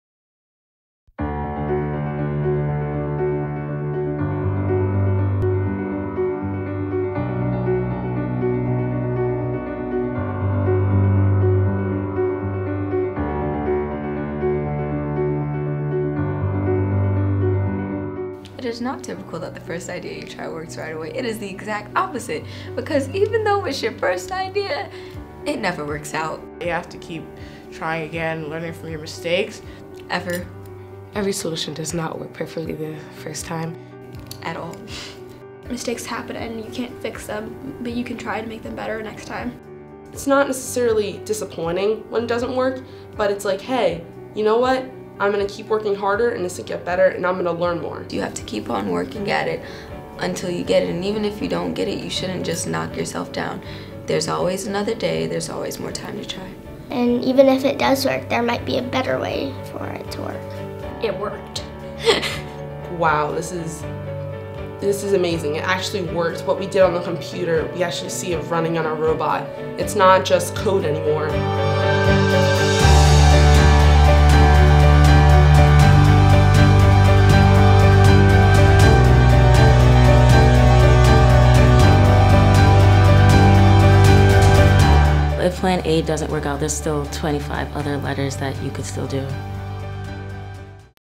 Put Her FIRST: Problem Solving
Ninety-eight percent of FIRST(R) participants say they improve their problem-solving skills. FIRST(R) LEGO(R) League gives girls the tools to build their confidence. and putherFIRST